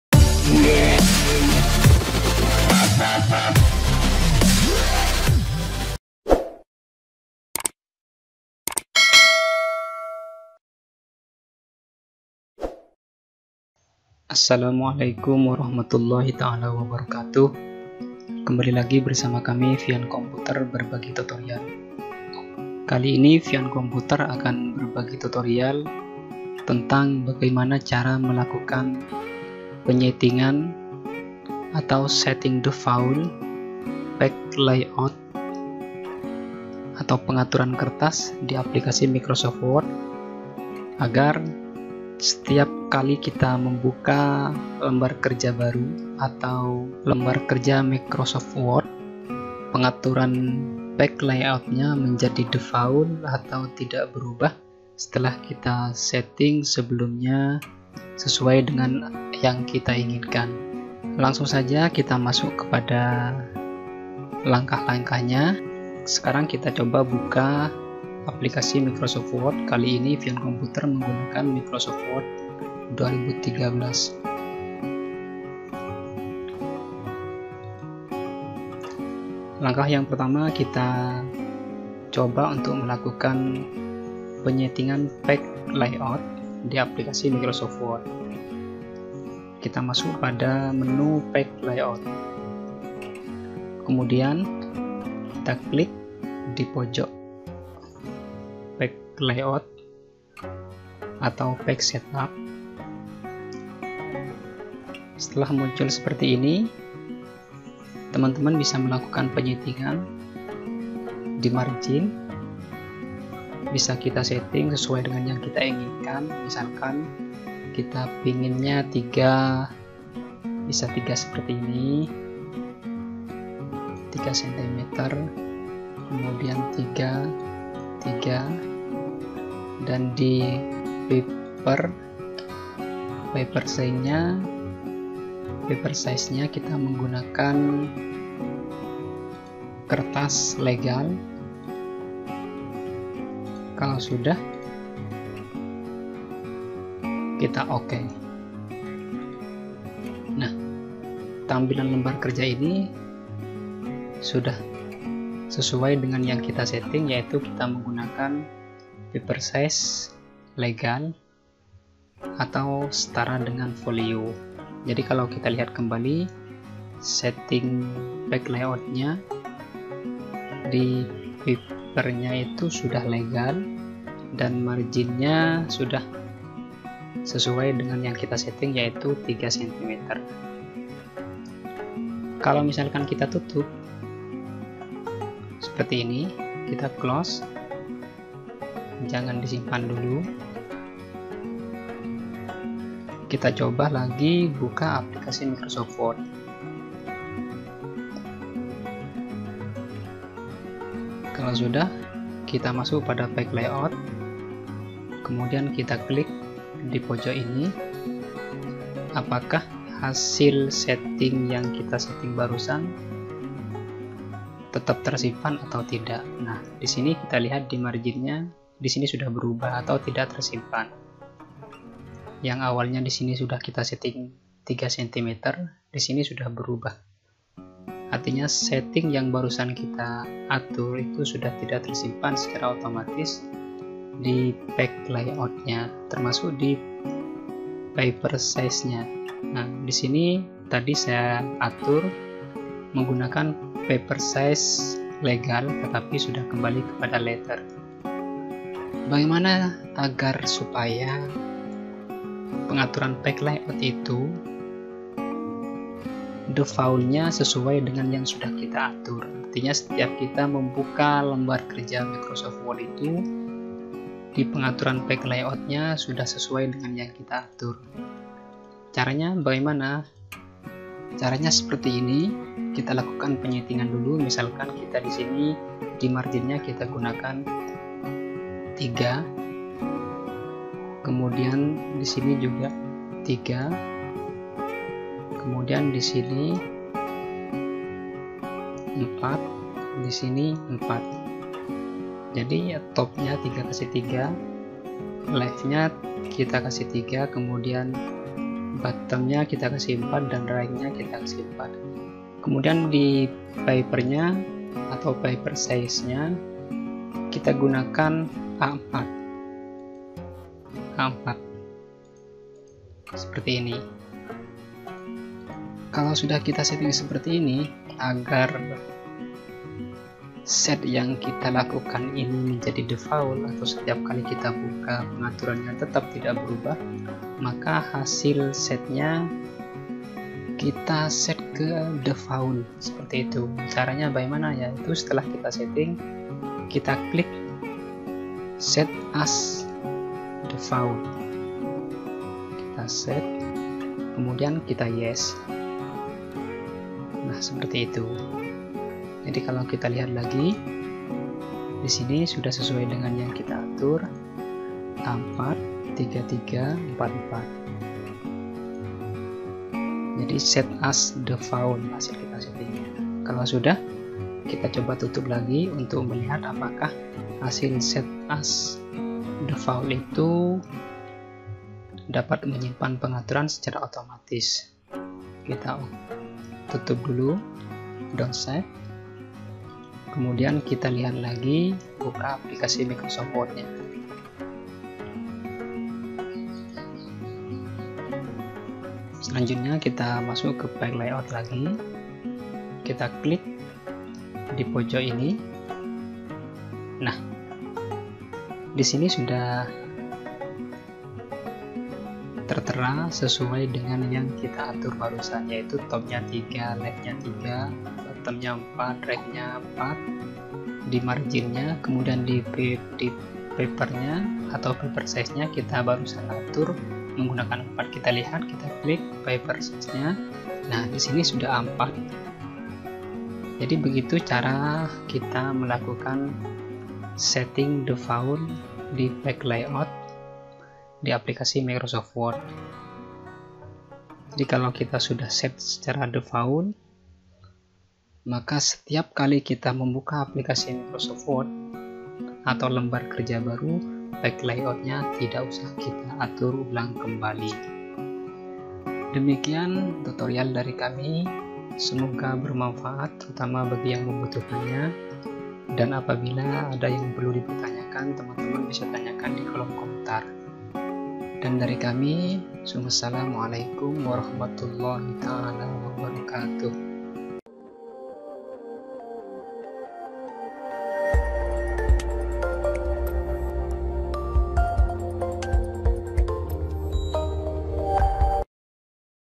CARA SETTING DEFAULT PAGE LAYOUT AGAR TIDAK BERUBAH | FIAN COMPUTER
Berbagi tutorial cara setting default page layout agar tidak berubah di microsoft word agar setiap kali kita membuka lembar kerja baru pengaturan page layout tidak berubah, tetapi tetap tersimpan sebagaimana yang sudah kita atur sebelumnya.